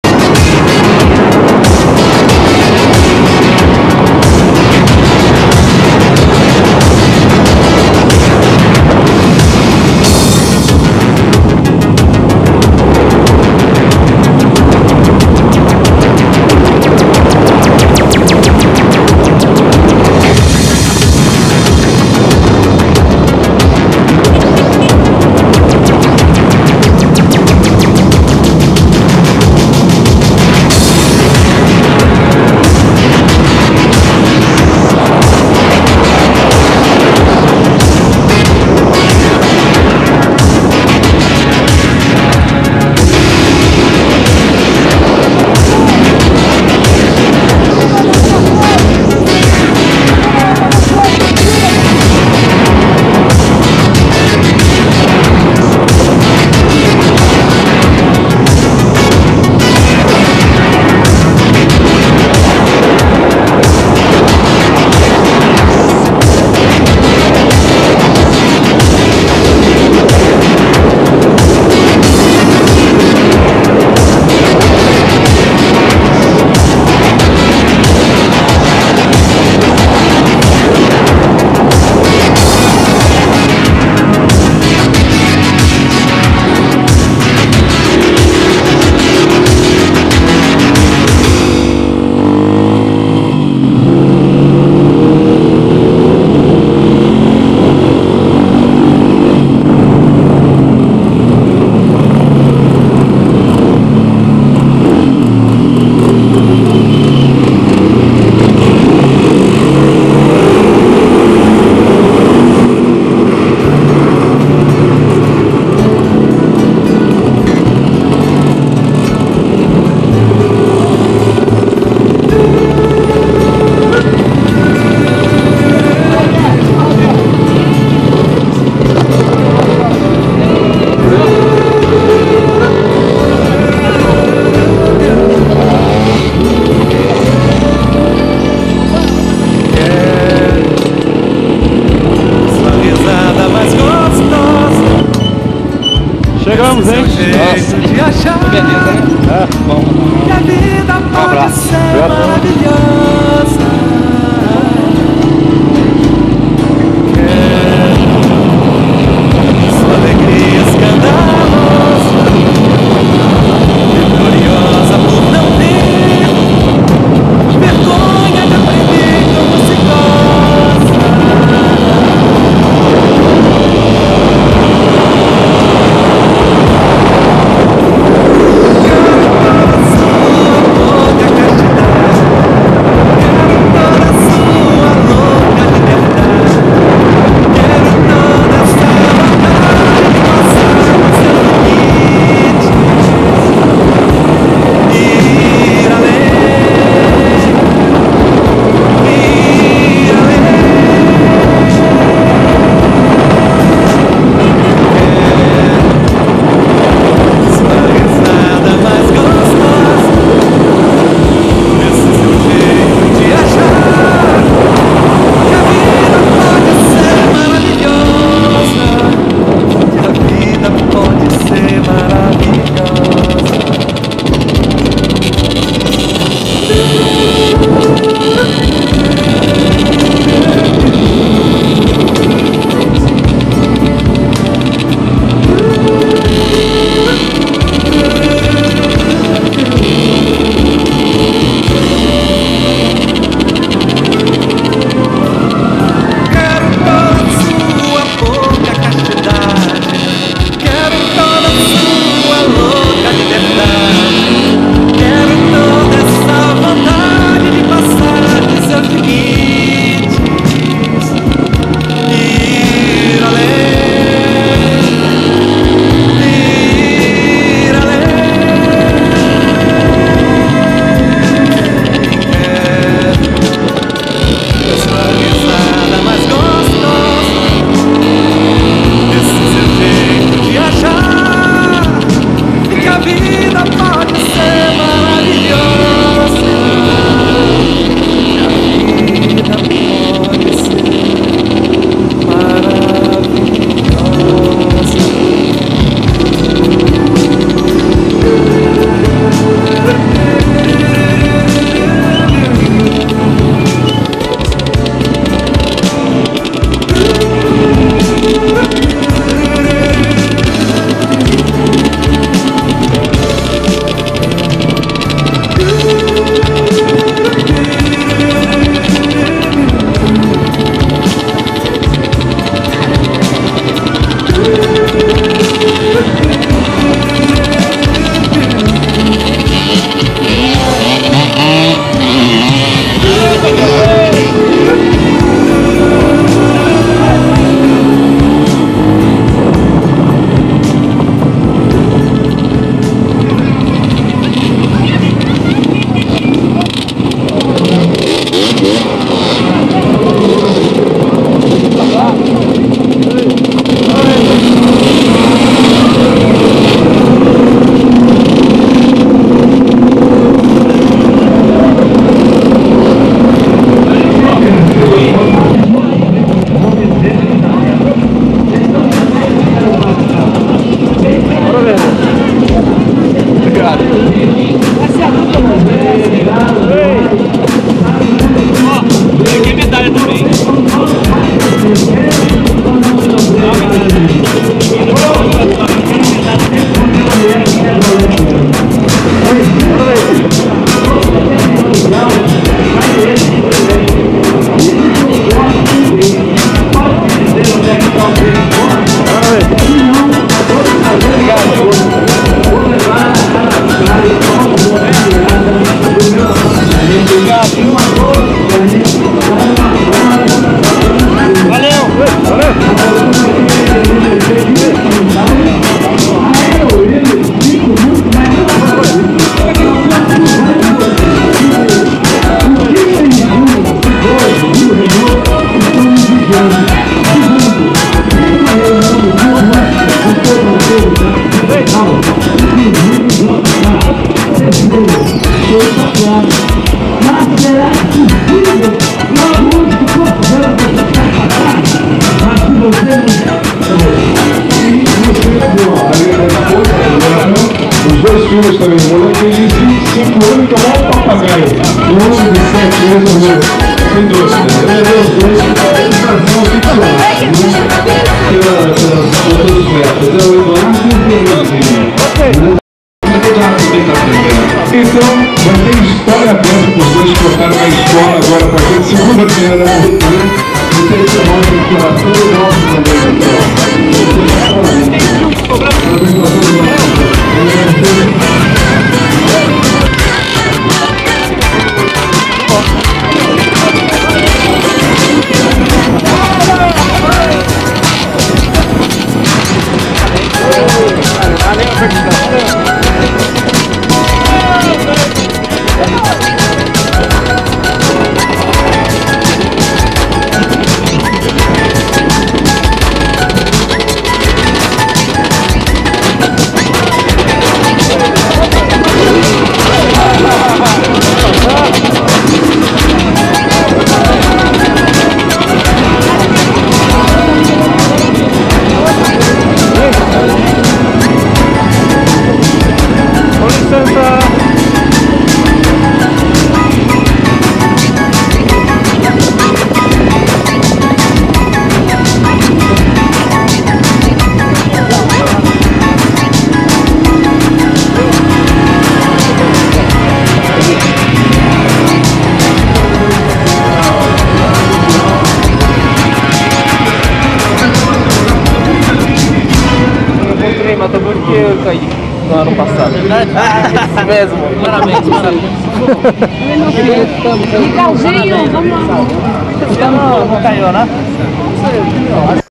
0085 VIII Enduro da Independencia 11-12
VIII Enduro da Independencia, Ubatuba→Belo Horizonte (Ver. Completo)
ブラジルの雄大な大自然の中を３日間にわたってオートバイで走りながら撮影した第８回独立記念日エンデューロ大会にの映像です。(完全版)